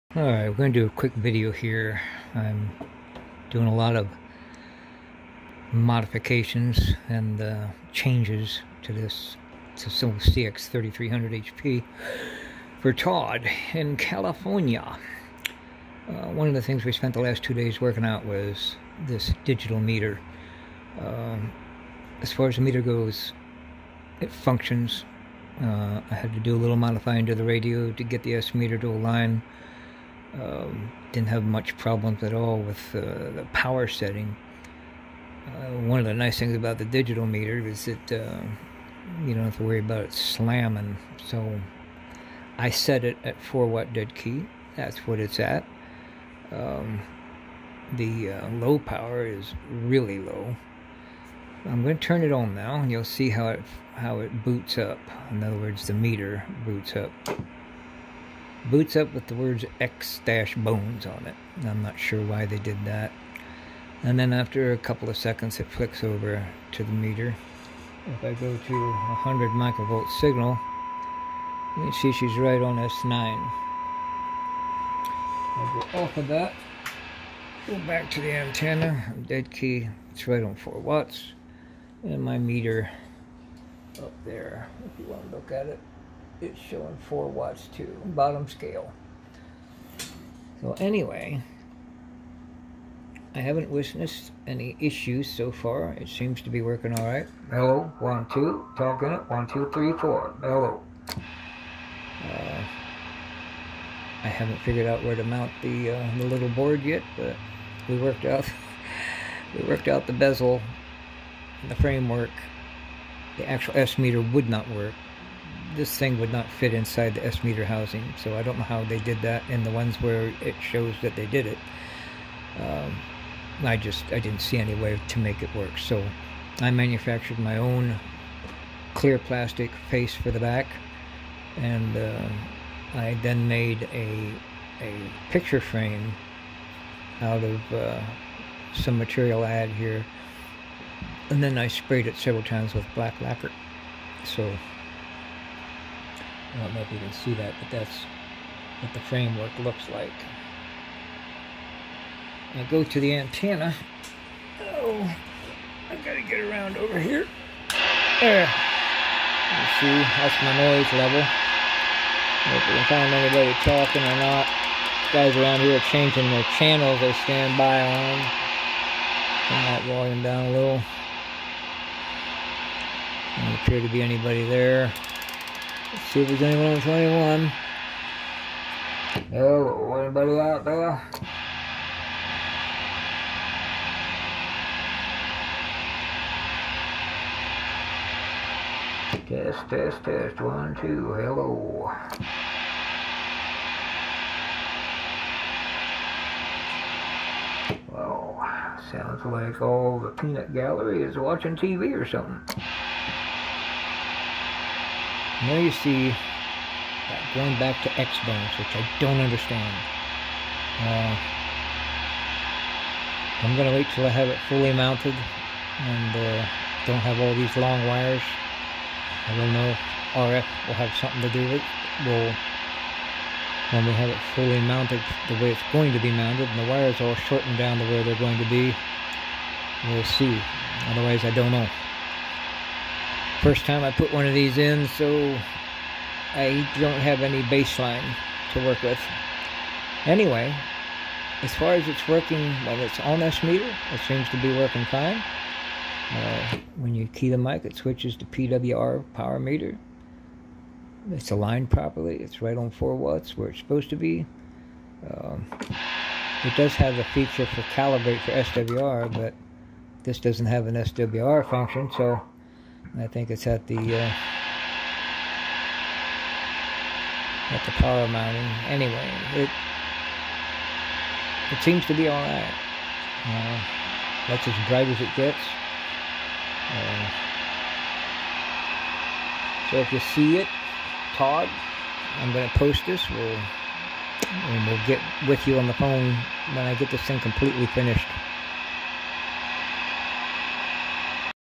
TODDS 3300HP DIGITAL METER INSTALL
This is Todd's radio in California we did a bunch of mods for him on this but the feature of this video is the S-Meter.